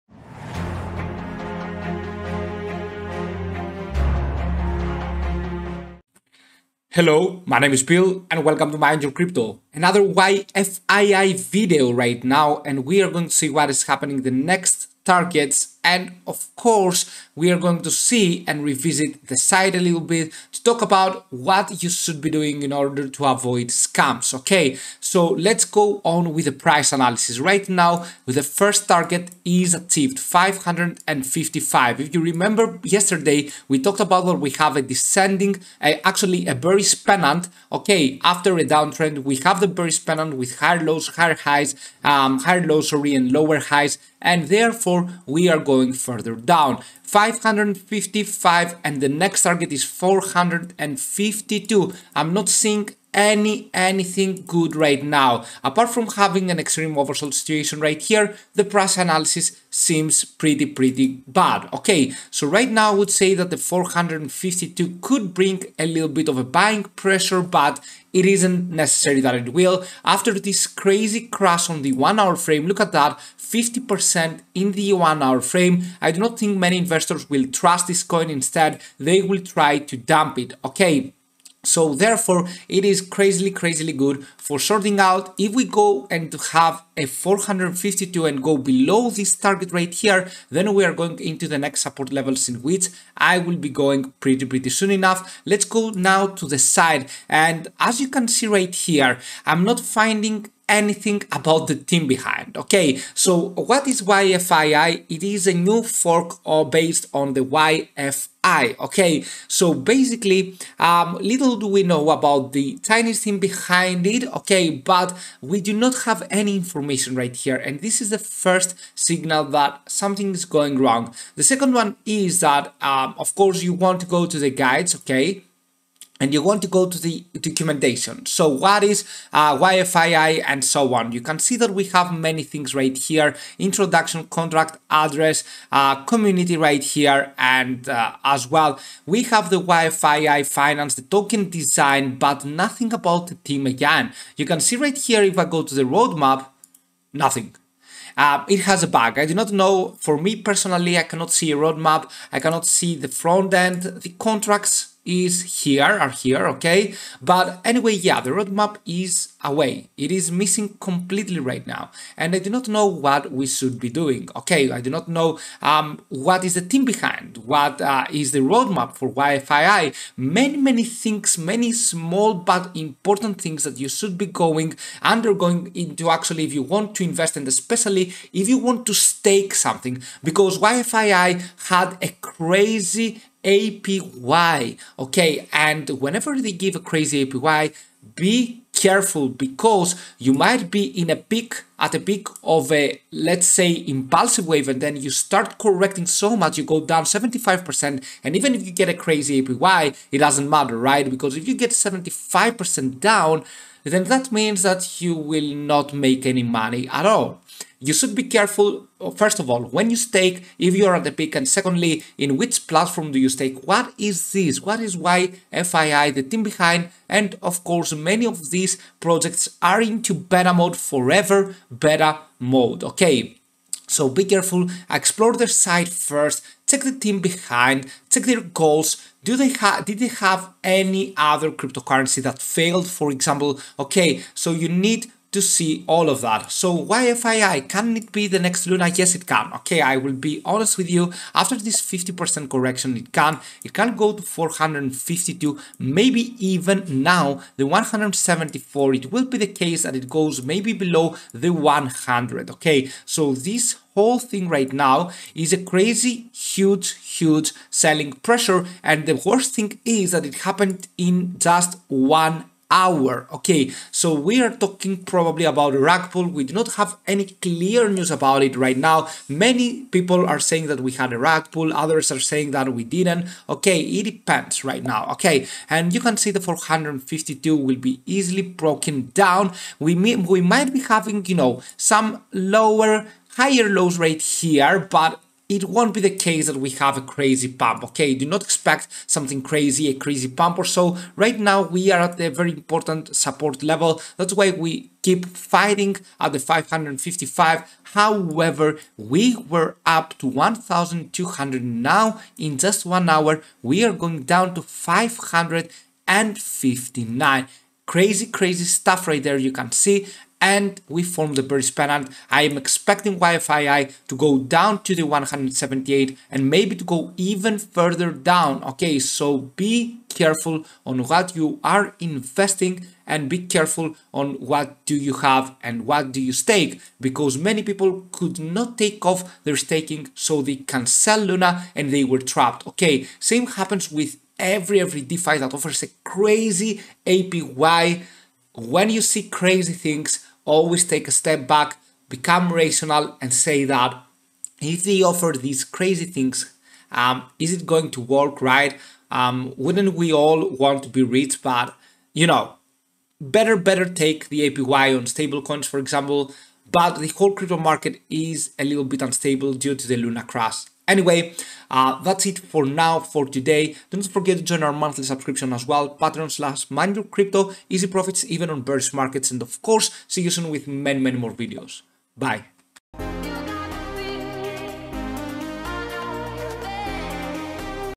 DFI.MONEY(YFII) GOING TO ZERO? How Can You Spot The NEXT LUNA!
In this video we will provide the news and price prediciton of the DFI.MONEY(YFII) IN USDT. What is happening? Is it going to recover? Where is the team behind? Are investors interested? We are going to answer all of these questions!

💰💰 JOIN PREMIUM